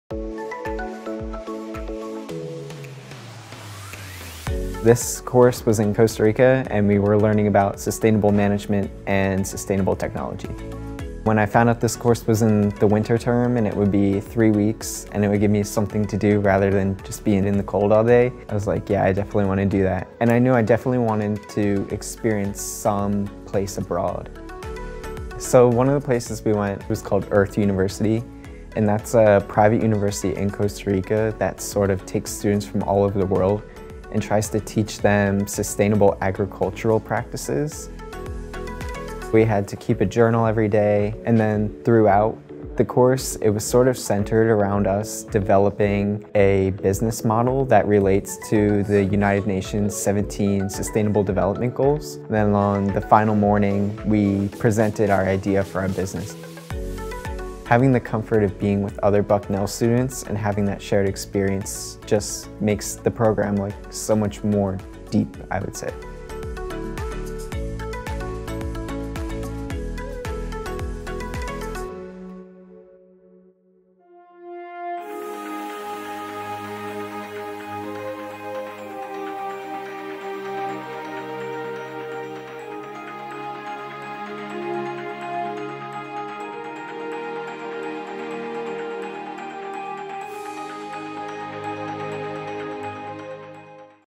Bucknell In: University Course 200 in Costa Rica
Bucknell students travel to Costa Rica to learn about sustainable management and technology in this three-week course.